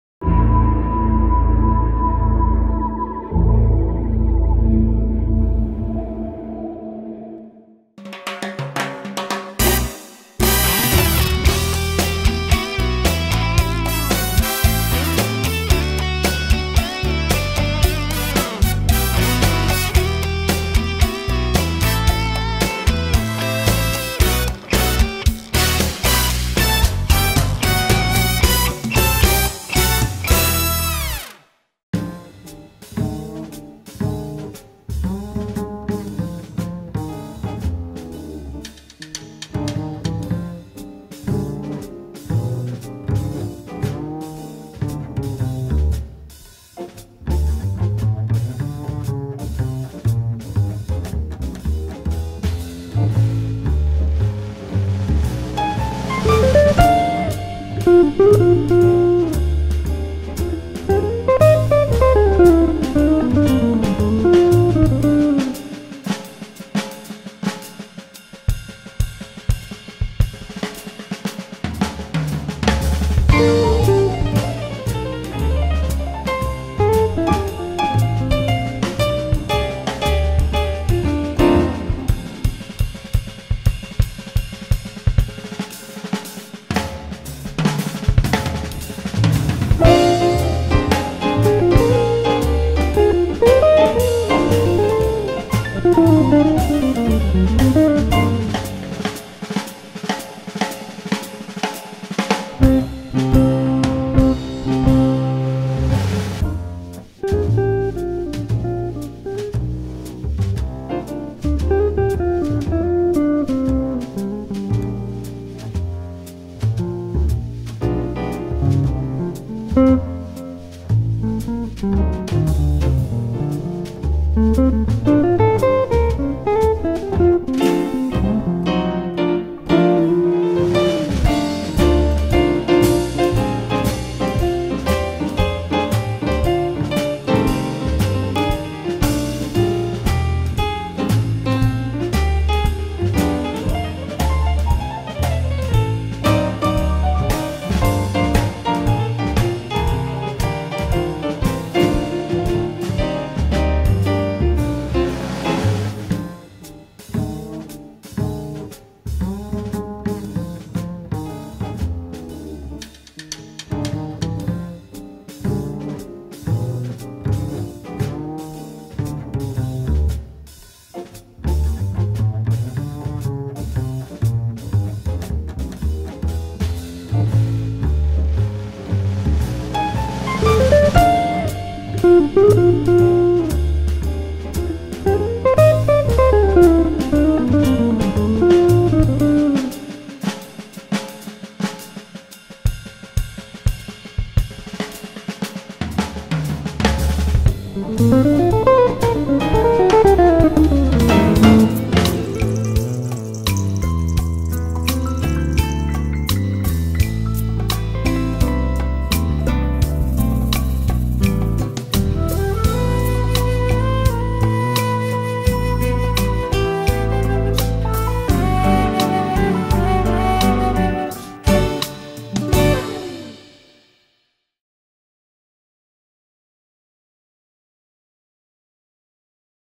LFX team mission on Debby two wreck dive video
LFX team on a mission to control agains invasive Lionfish species in Aruban waters. Area is relatively clear. Is a good uneventful dive so we have some fun checking out the scenery.